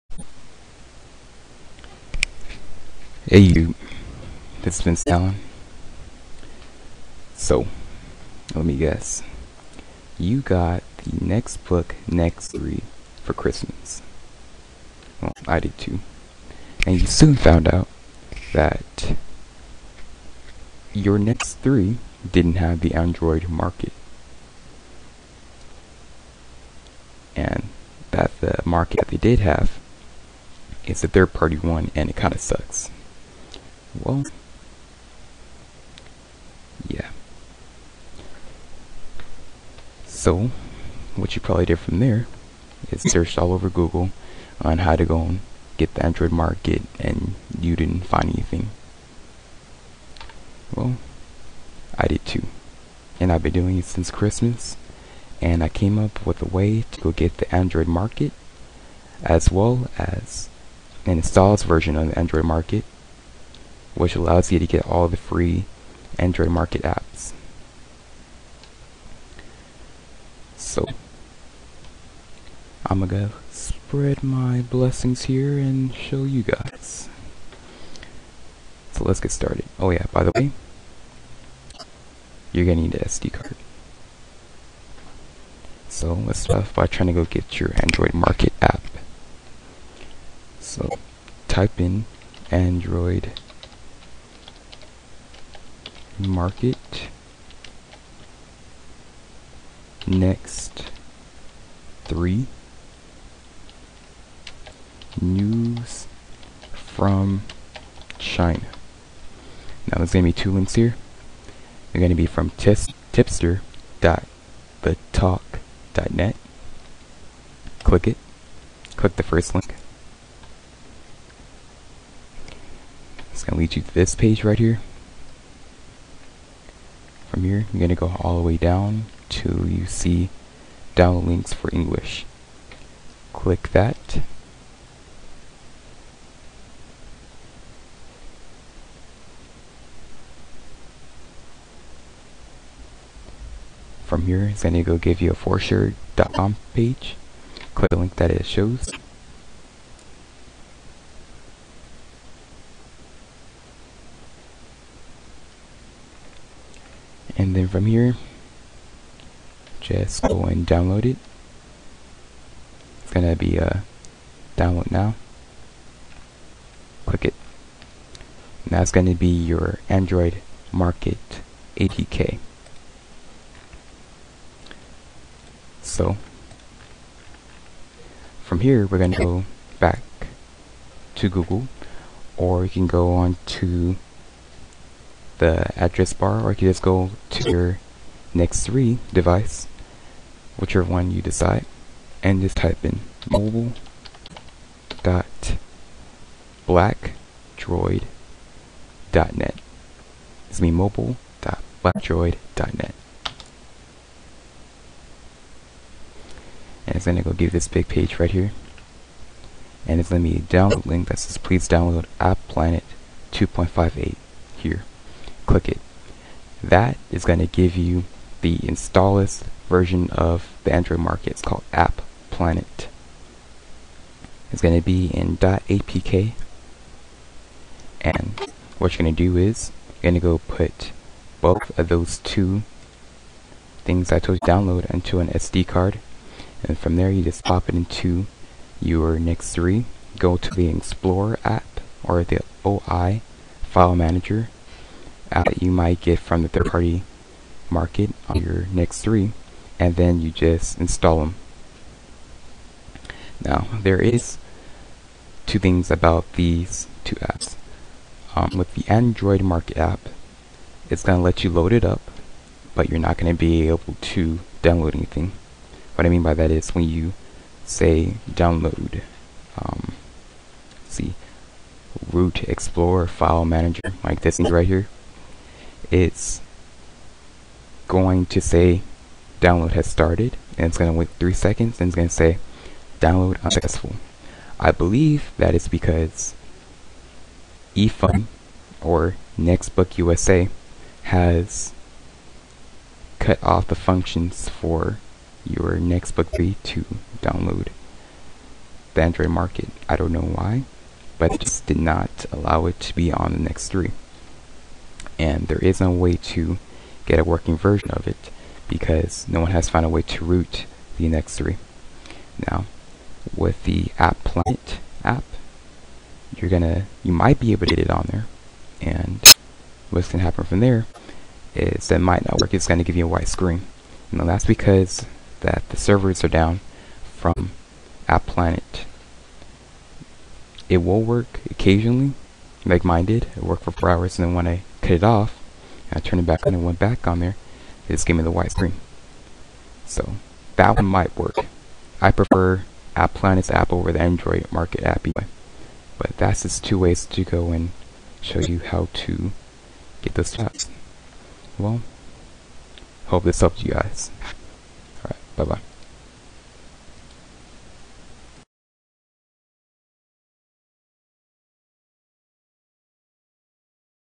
NEXT3: HOW TO GET THE ANDROID MARKET AND APPLANET ON YOUR NEXTBOOK NEXT3 TABLET!!! NO ROOTING
*UPDATE!* okay before any of you start complaining, Applanet servers are always being shutdown then turned on through various times, i stopped trying to keep up with them. i now use Snappzmarket(google it on your phone, click the 1st link and it should automatically download), its reliable but i still prefer the ui of Applanet. you can always keep the app and check on it occasionally. like i said they always randomly get new servers, but Applanet is usually down for several months at a time*

Like the title says, I found a way to get the android market on your NextBook Next3 tablet, no rooting needed!

sorry for the audio guys, my mic is a little glitchy =)

Applanet is the installous version of the android market, it lets you download apps and games from the market for free

requirements: Nextbook next3
                          Sd card (2gbs or higher is recommended
                          Computer or Laptop

Hopes this helps! thumbs up if you liked!!!